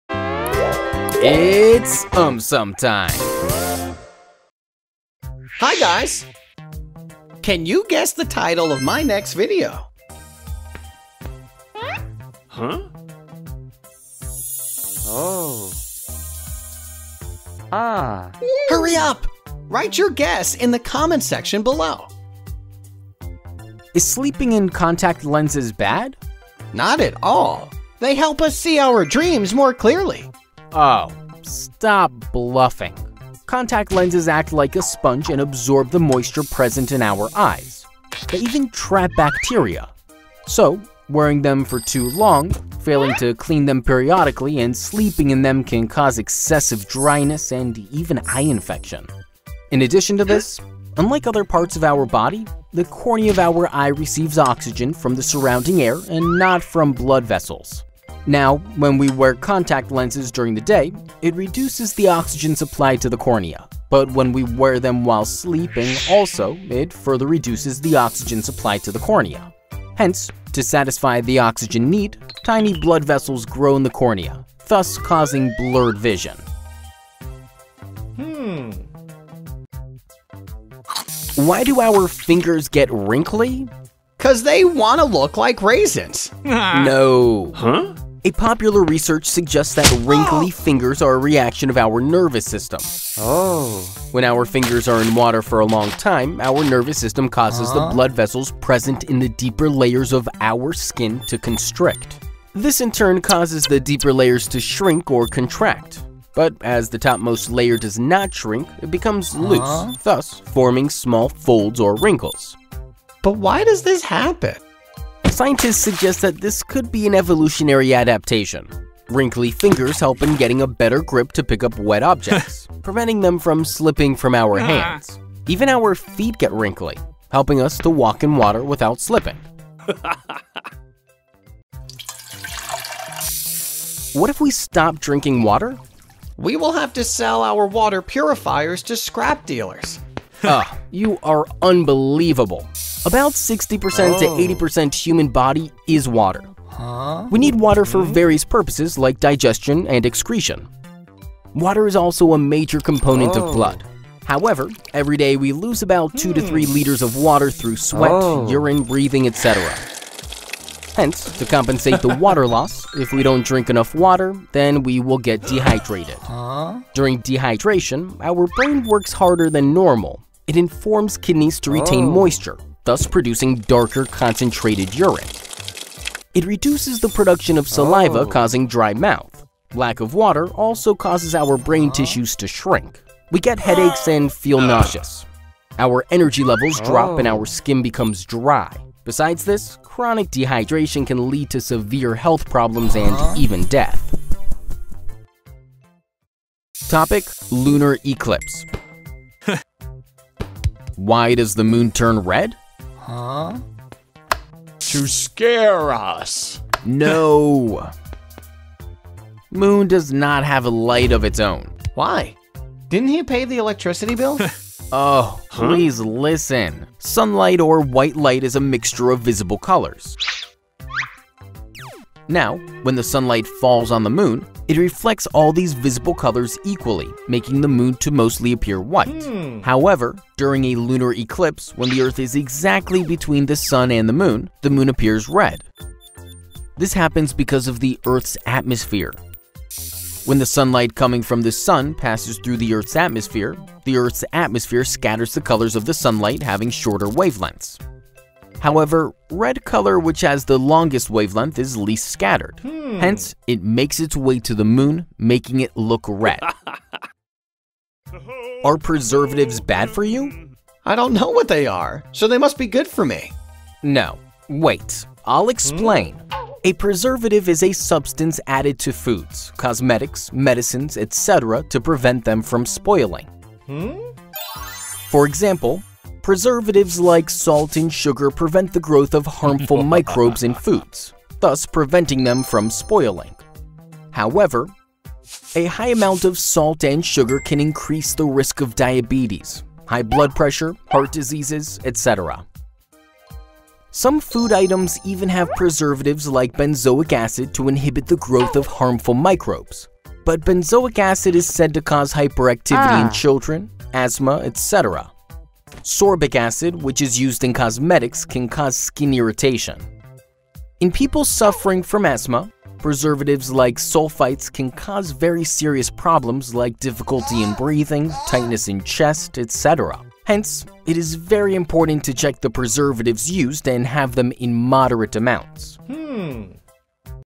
Is sleeping in Contact Lenses bad? + more videos
Contact lenses act like a sponge and absorb the moisture present in our eyes. They even trap bacteria. So, wearing them for too long, failing to clean them periodically and sleeping in them can cause excessive dryness and even eye infection.

In addition to this, unlike other parts of our body, the cornea of our eye receives oxygen from the surrounding air and not from blood vessels.

Now, when we wear contact lenses during the day, it reduces the oxygen supply to the cornea. But when we wear them while sleeping also, it further reduces the oxygen supply to the cornea. Hence, to satisfy the oxygen need, tiny blood vessels grow in the cornea, thus causing blurred vision.